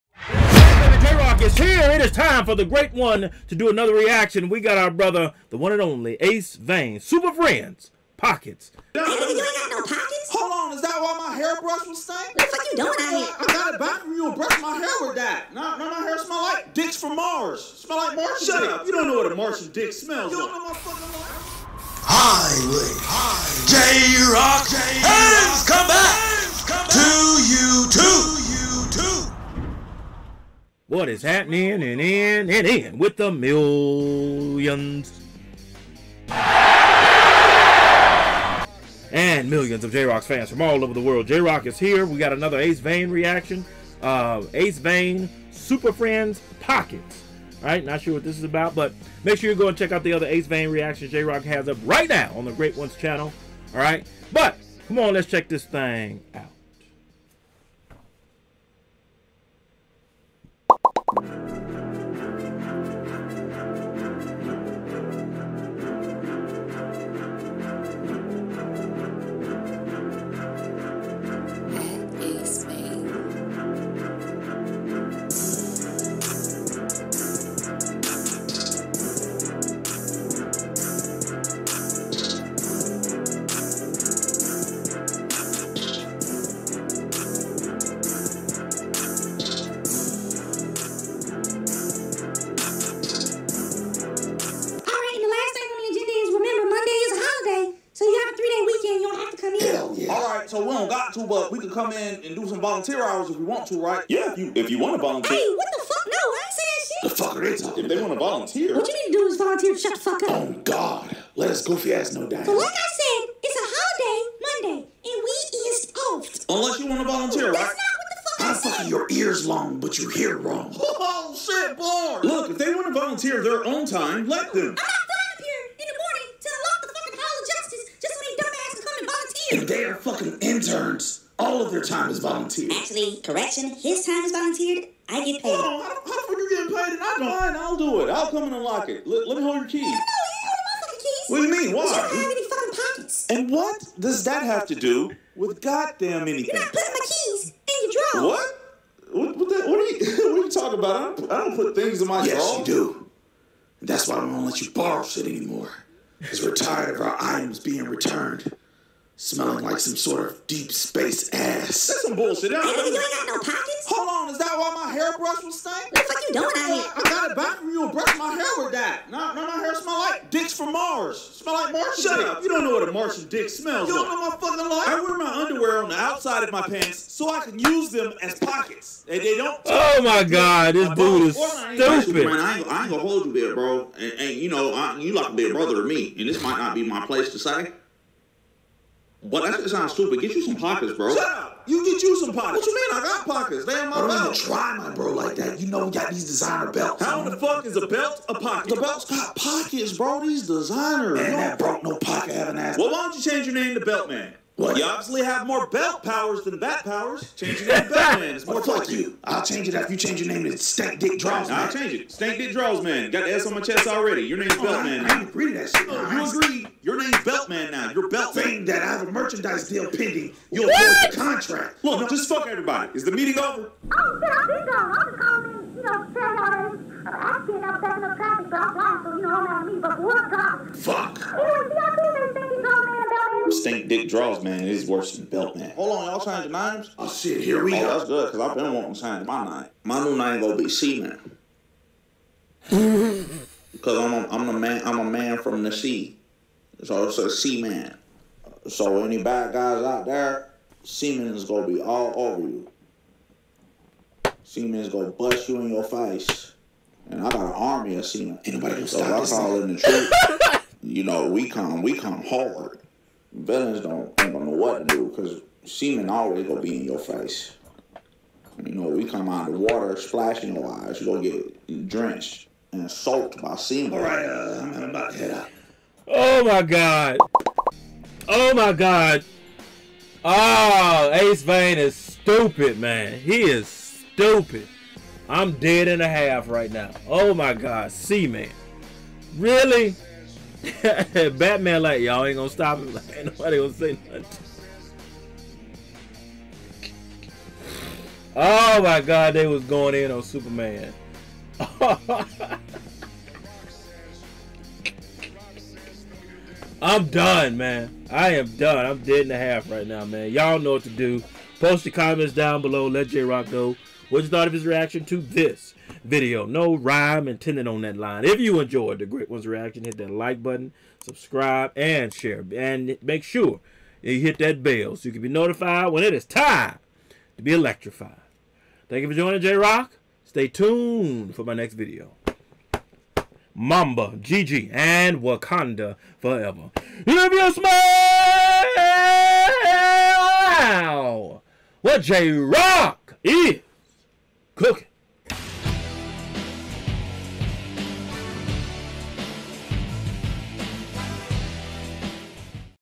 Acevane SuperFriends "Pockets" Reaction 😂😂😂
J-Rocc The Gr81 Reacts to another Hilarious video by Acevane SuperFriends "Pockets". Acevane is Hilarious!!!